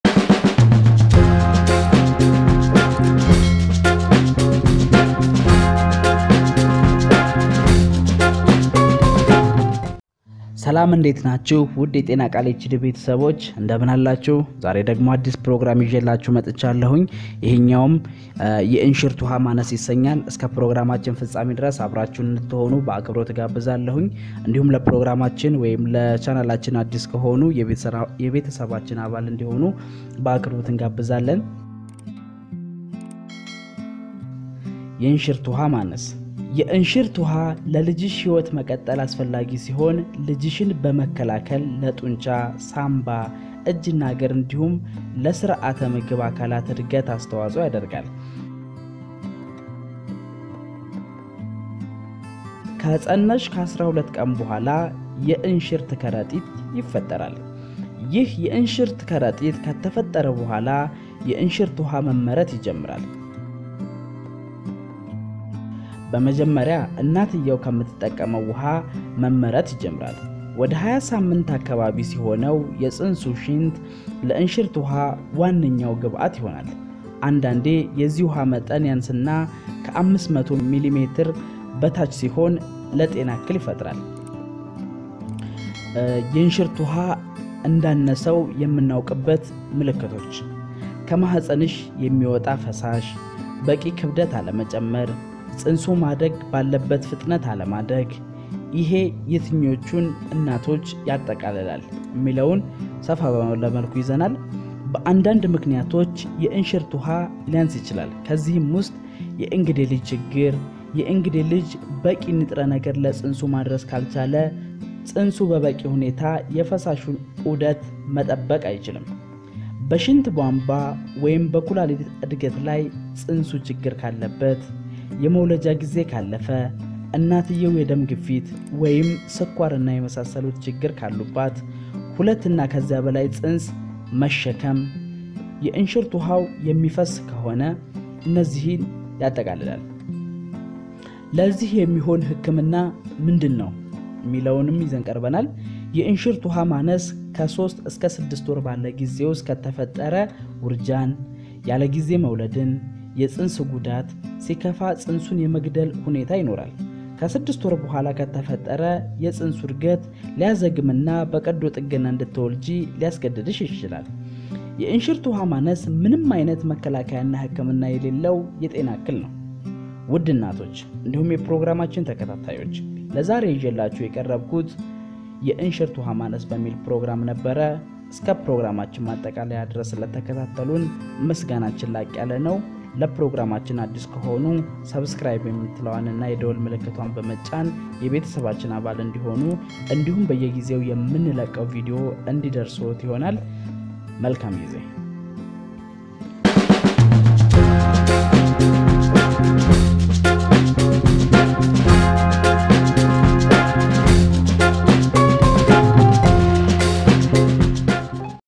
የእንሽርት ውሃ ማነስ || Amniotic fluid a miracle
የእንሽርት ውሃ ማነስ
የእርሽርት ውሃ ለልጅሽ ህይወት መቀጠል አስፈላጊ ሲሆን ልጅሽን በመከላከል ለጡንቻ ፤ሳምባ፤ እጅና እግር፤ እንዲሁም ለስርአተ ምግብ አካላት እድገት አስተዋፅዎ ያደርጋል፡፡ ከፀነስሽ ከ 12 ቀን በኋላ የእርሽርት ከረጢት ይፈጠራል፡፡ ይህ የእርሽርት ከረጢት ከተፈጠረ በኋላ የእርሽርት ውሀ መመረት ይጀምራል፡፡ በመጀመሪያ እናትየው ከምትጠቀመው ውሀ መመረት ይጀምራል፡፡ ወደ 20 ሳምንት አካባቢ ሲሆነው የፅንሱ ሽንት ለእንሽርት ውሀ ዋንኛው ግብዓት ይሆናል፡፡ አንዳንዴ የዚህ ውሃ መጠን ያንስና (ከ 500 ml በታች ሲሆን) ለጤና እክል ይፈጥራል።